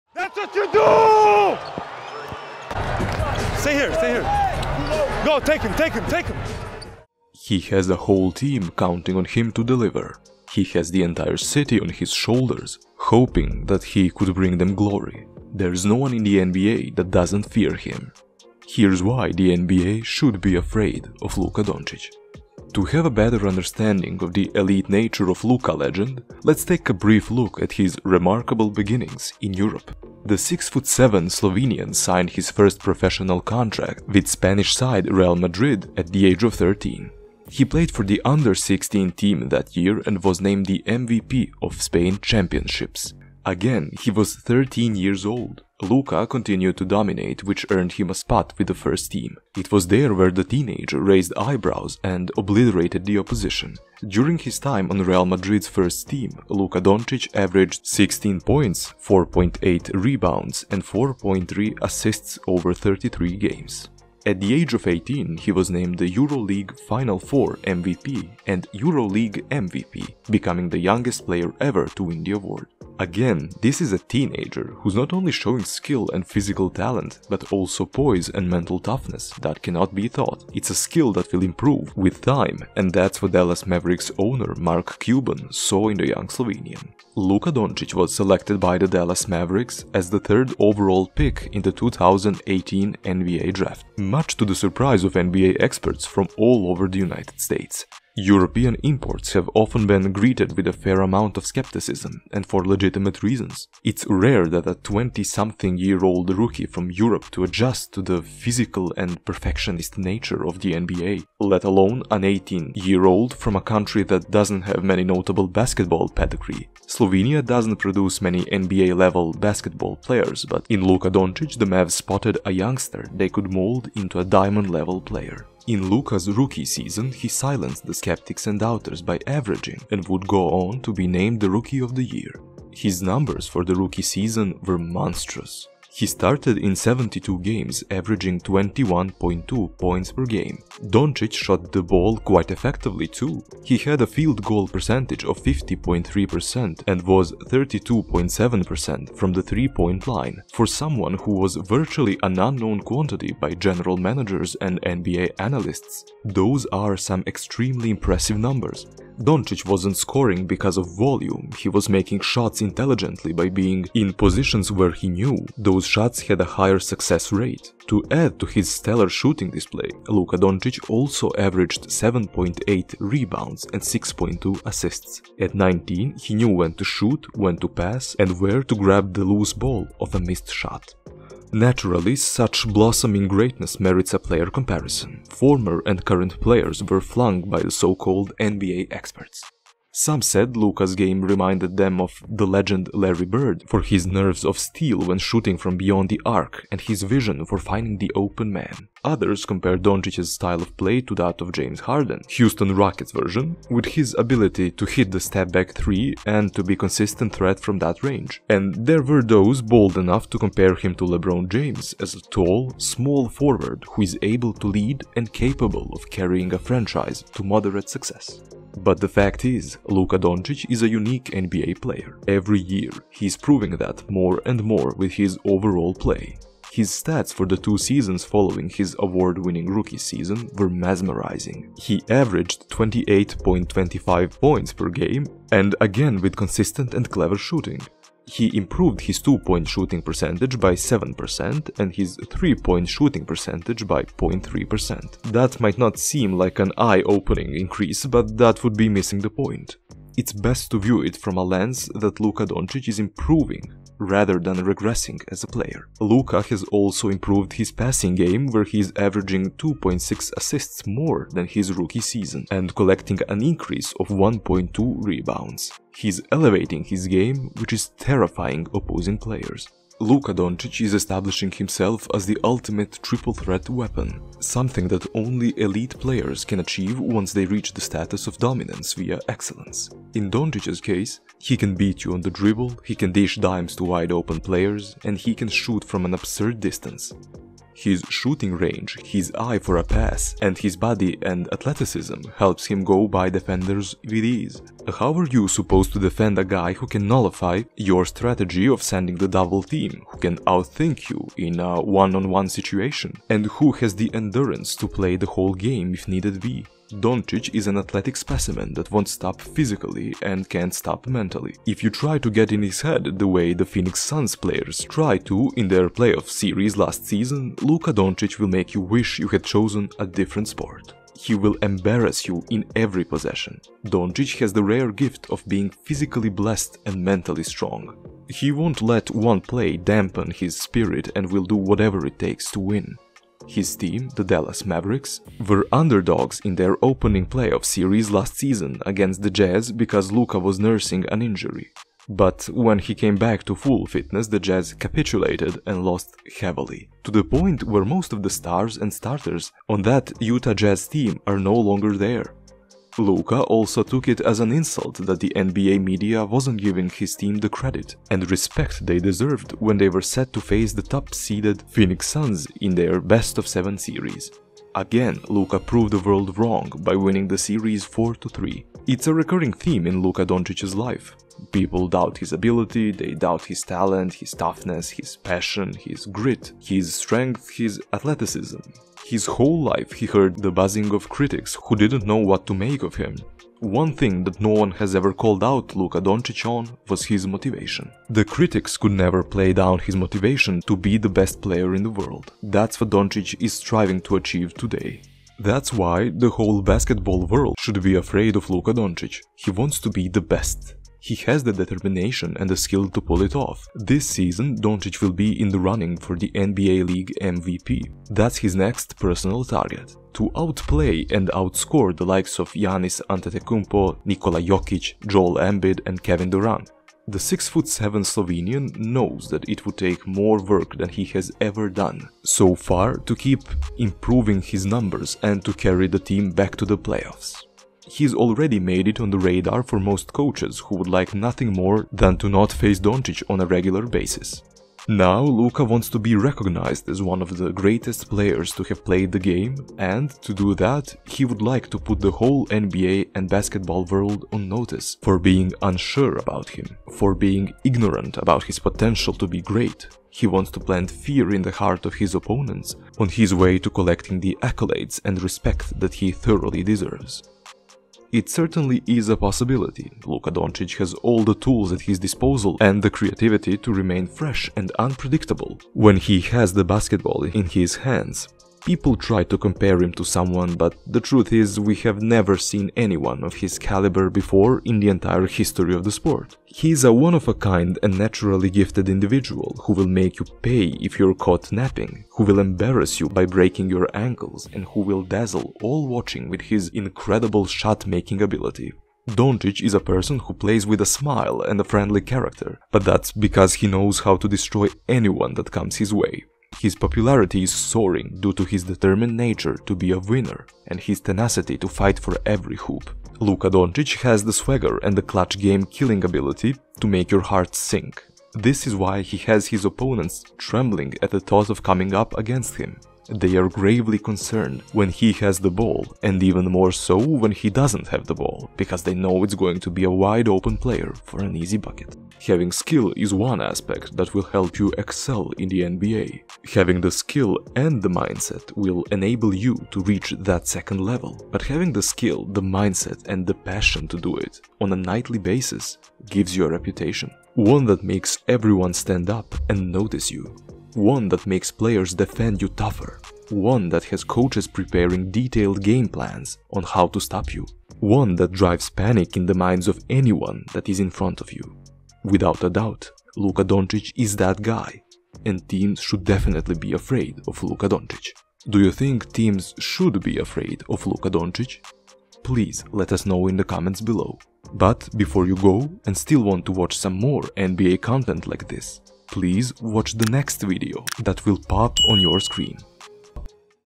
How Luka Doncic Took over The NBA
They NBA is Afraid of Luka Doncic and this is WHY.